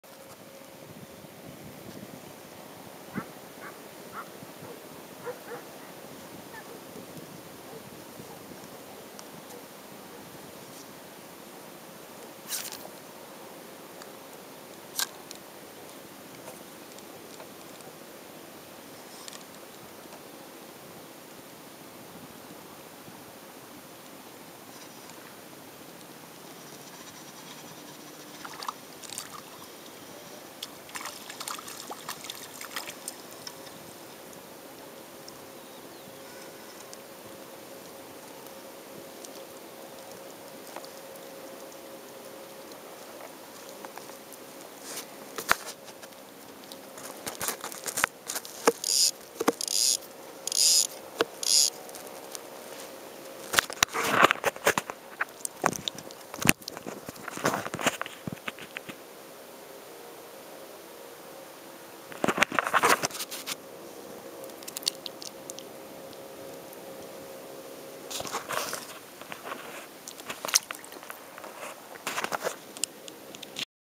Light Rock Fishing (LRF) for gilt-head seabream / lidaki çipura, Izmir - Turkey
Having Friday evening light game fun after work, Izmir-Turkey. Recorded using Pentax W90.
-Bait: Marukyu Power Isome L (natural brown),
-Jighead: Ecogear Shirasu Head Fine 0.6gr #10,
-Line: Sunline Light Game 0.4 PE + 0.17mm FC leader,
-Rod: Promarine Salty Rugger 73L 0.5-7gr,
-Reel: Shimano Sienna 1000RD.